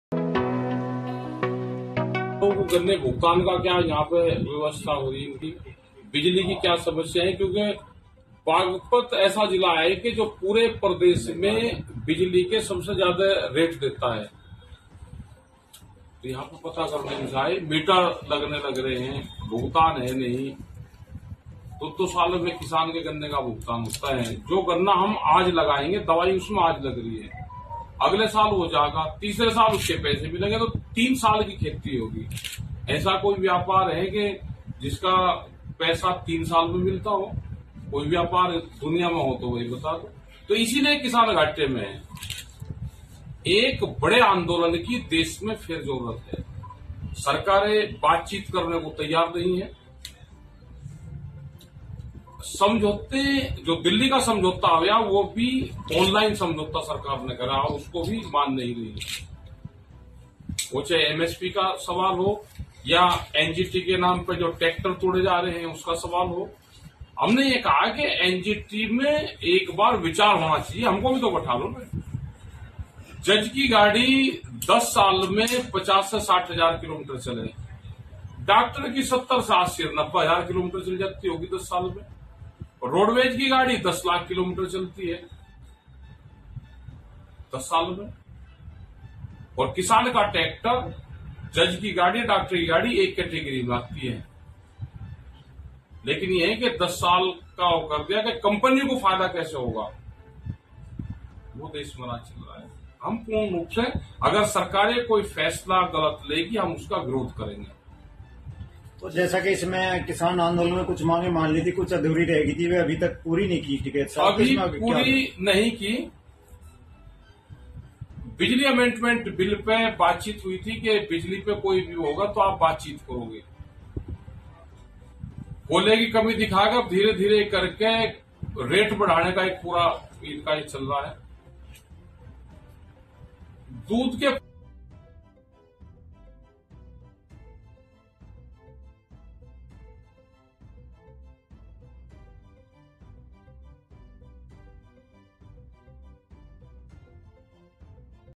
'Modi सरकार का मेनिफेस्टो झूठा, देश में एक और बड़े आंदोलन की जरुरत...'- Rakesh Tikait
भाकियू का नया संगठन बनाए जाने को लेकर भाकियू के राष्ट्रीय प्रवक्ता राकेश टिकैत ने चुप्पी तोड़ी है। उन्होंने कहा कि नया संगठन बनने पर भाकियू पर कोई फर्क नहीं पड़ेगा। पहले भी संगठन से लोग जाते रहे हैं। बता दें कि पश्चिमी उत्तर प्रदेश के खाप चौधरियों ने किसानों की समस्याओं के समाधान के लिए 17 अक्तूबर 1986 को संगठन की नींव रखी थी, लेकिन पिछले 32 सालों में समय समय पर संगठन में बिखराव और फूट देखने को मिली।